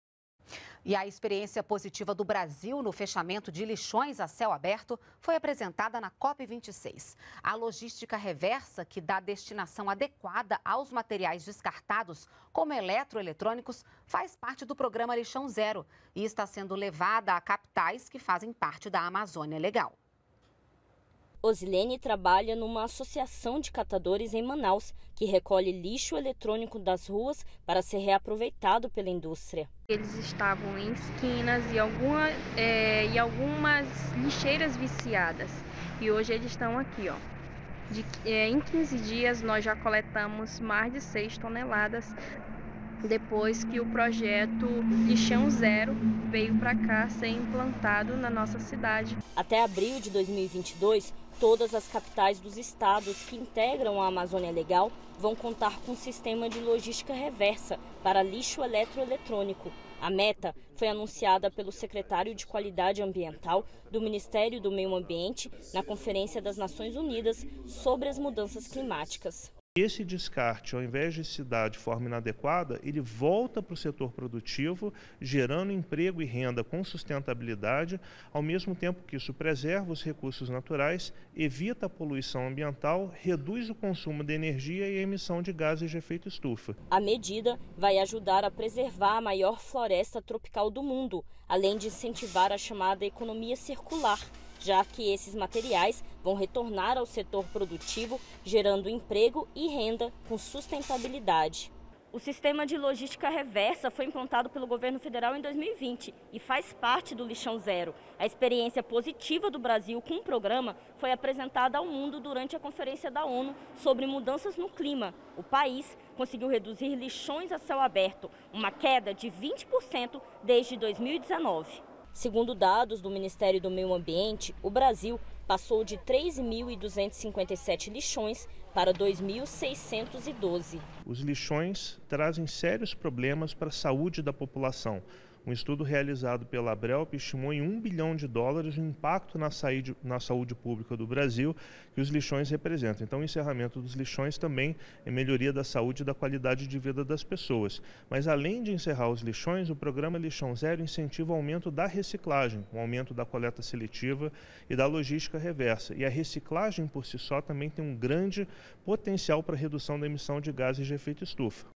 Lixão Zero
09.11.21 - A experiência positiva do Brasil no fechamento de lixões a céu aberto foi apresentada na COP26. A Logística Reversa, que dá destinação adequada aos materiais descartados, como eletroeletrônicos, faz parte do Programa Lixão Zero e está sendo levada às capitais que fazem parte da Amazônia Legal.